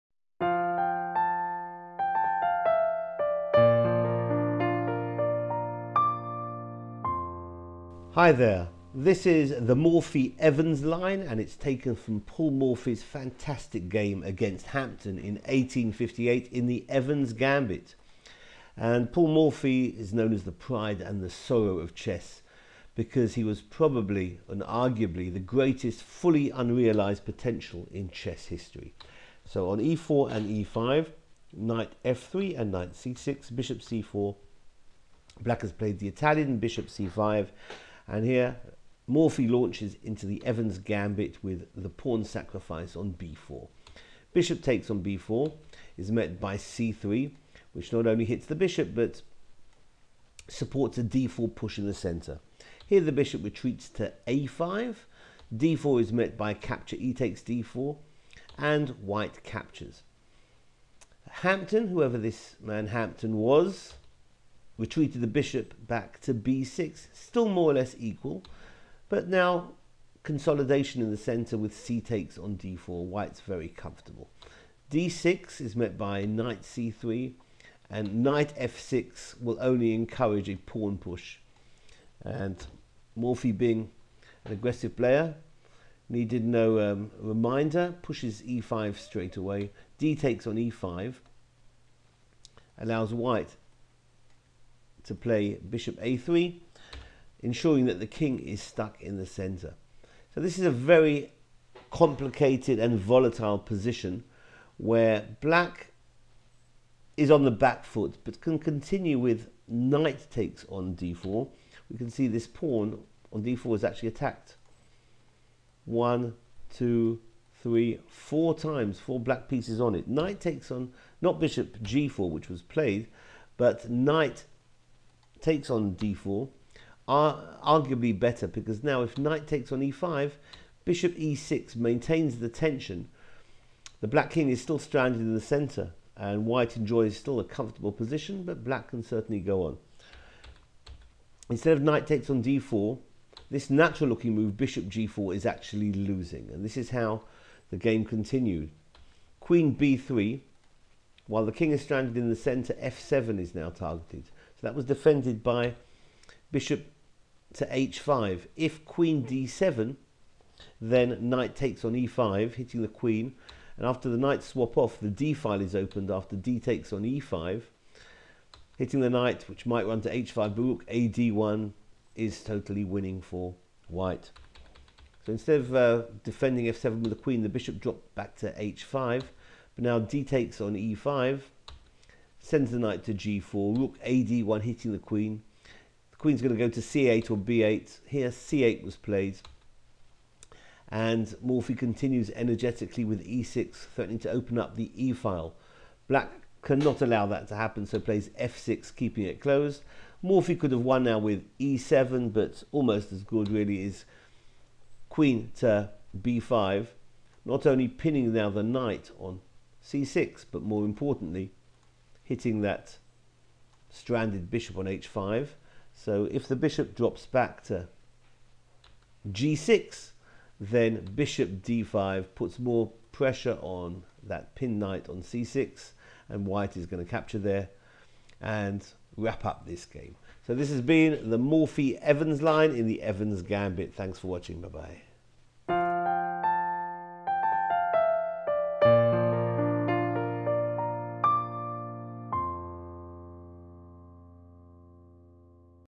Evans Gambit - The Morphy Evans Line (trap no. 83)
1.e4 e5 2.Nf3 Nc6 3.Bc4 Bc5 4.b4 Bxb4 5.c3 Ba5 6.d4 exd4 7.0–0 Bb6 8.cxd4 d6 9.Nc3 Nf6 10.e5 dxe5 11.Ba3 Bg4 12.Qb3 Bh5 13.dxe5 Ng4 14.Rad1 Qc8 15.e6 f6 16.Qb5 Bg6 17.Bd5